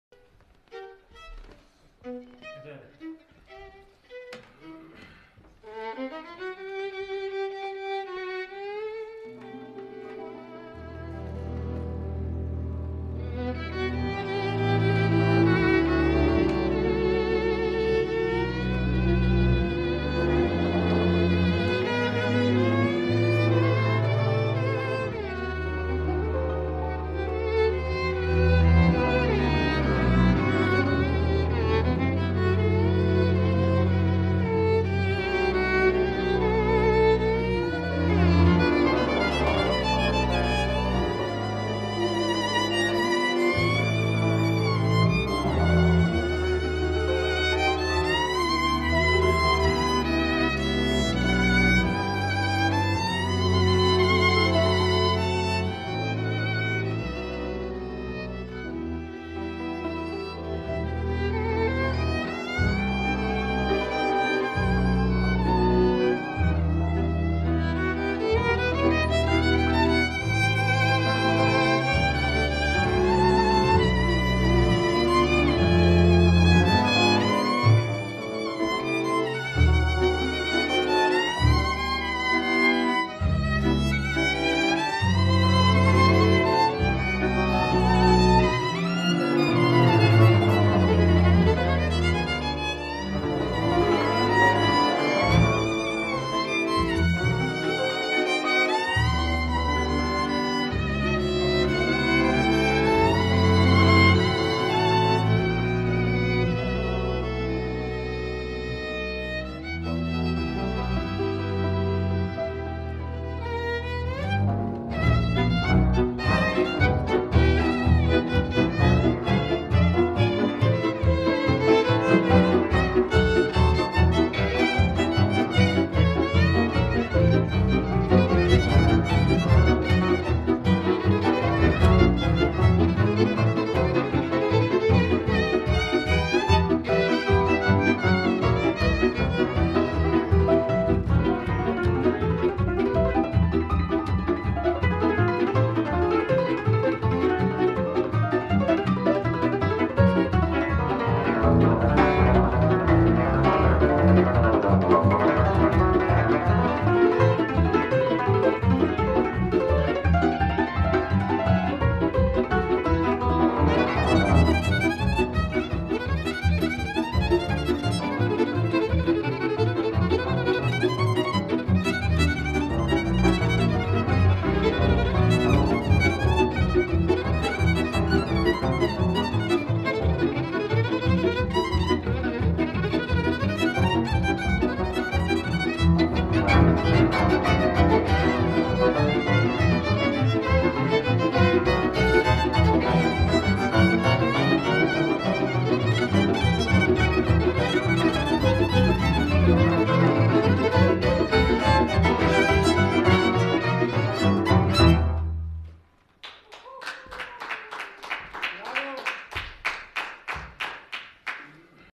Ivan Herák & band:Ruské cigánské piesne - vaša youtube hudba
Ivan Herák je špičkový slovenský huslista.Jeho hra sa vyznačuje vyskokým hudobným cítením v kombinácii s dokonalým technickým prevedením.